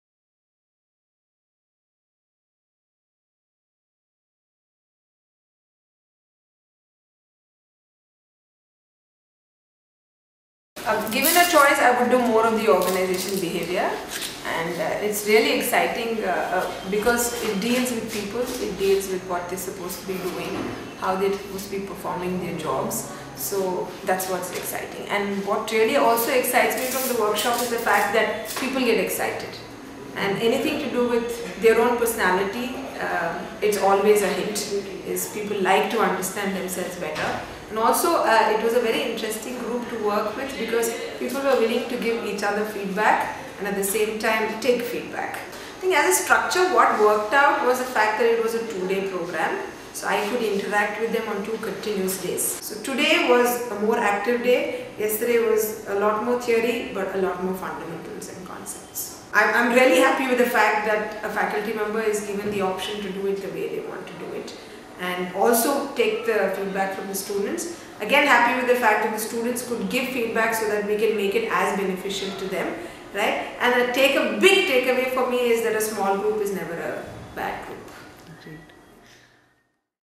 Padma Srinivas: A testimonial on Pivoting organisational dynamics
Padma Srinivas speaks about her experience taking an Adianta workshop.